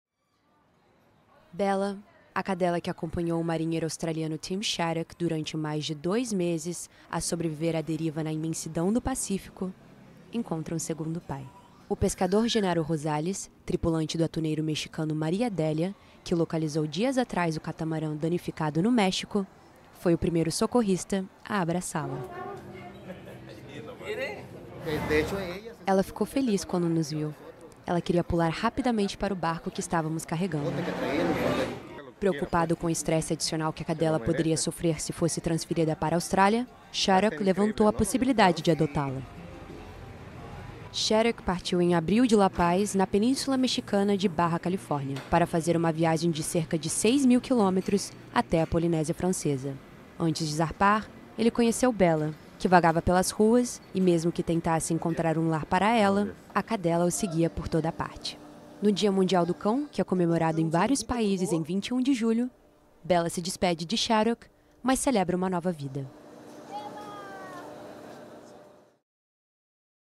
Bella comemora nova vida com 'segundo pai' no México após meses à deriva no Pacífico | AFP
A cadela que sobreviveu mais de dois meses no mar com peixe cru e água da chuva com o marinheiro australiano Tim Shaddock, encontra um ‘segundo pai’, nesta sexta-feira (21), que se comemora o Dia Mundial do Cão em diversos países. O pescador, Genaro Rosales, um dos primeiros a resgatá-la, lhe oferece um novo lar.